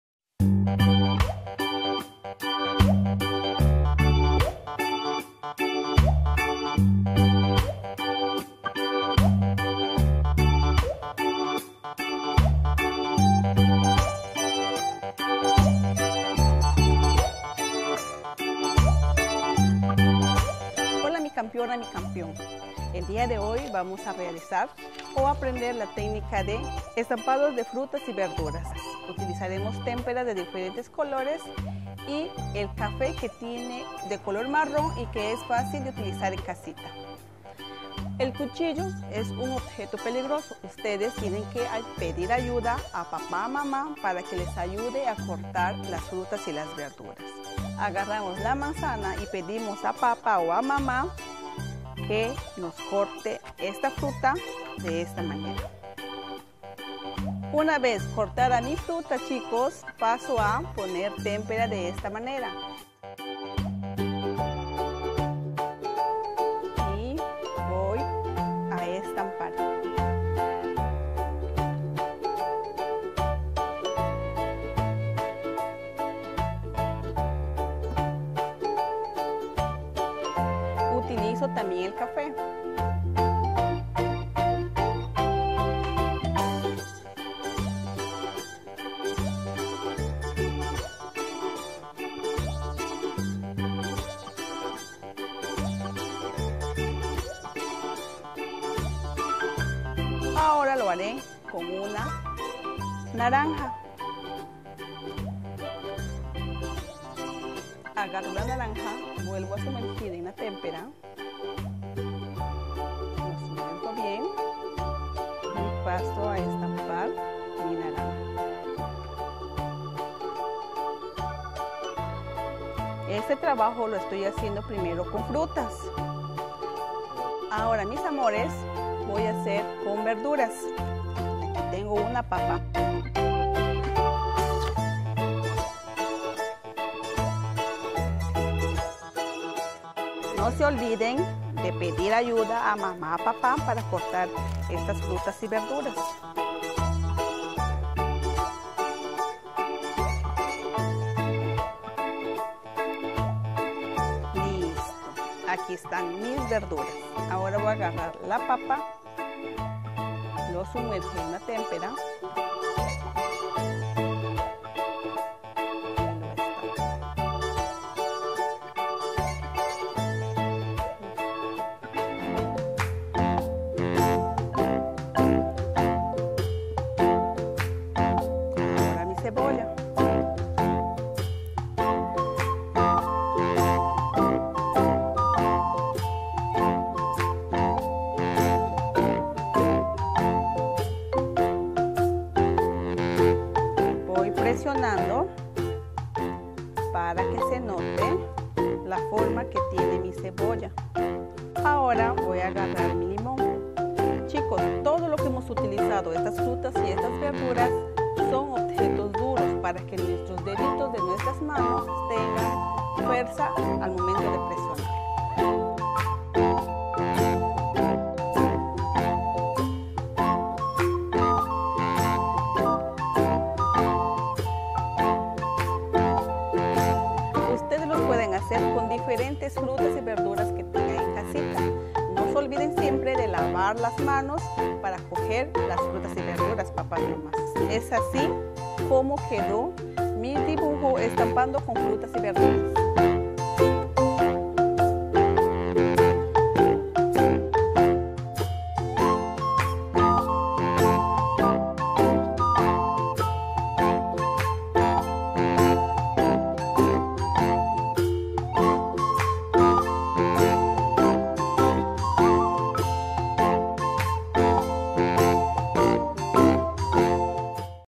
ESTAMPAMOS FRUTAS Y VERDURAS - MI UNIVERSO INFANTIL
UNA TÉCNICA MUY DIVERTIDA PARA REFORZAR NUESTROS DEDITOS CON MATERIALES SÓLIDOS. ¡A DIVERTIRNOS!